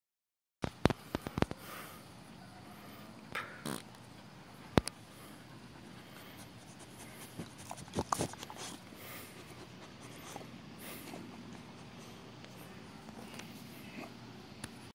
Run Like The Wind
13 year old chow Elsa is so fast